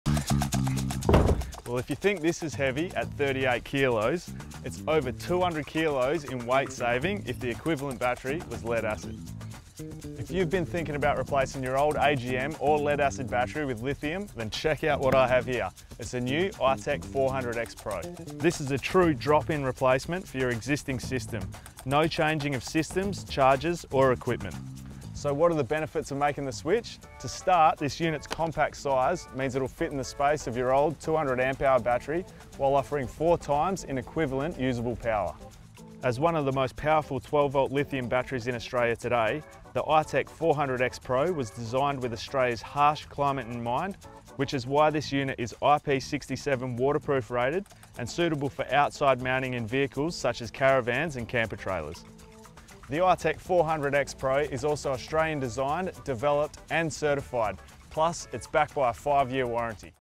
ITechworld lithium battery
Check out one of the most powerful 12v lithium batteries in Australia!